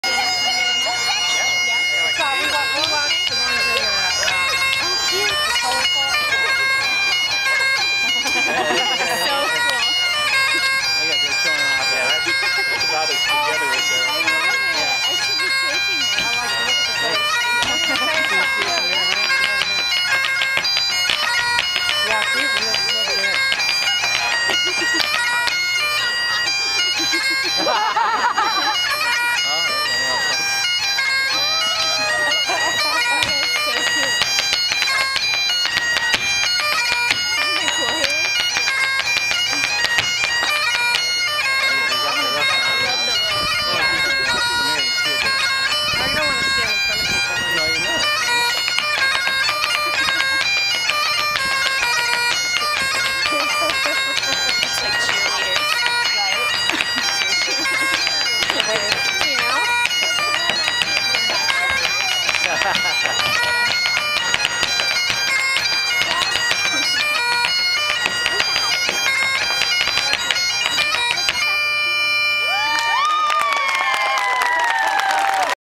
Irish vs. Scottish Jig
Irish and Scottish 'dance off' at the Monterey Highland Games 09.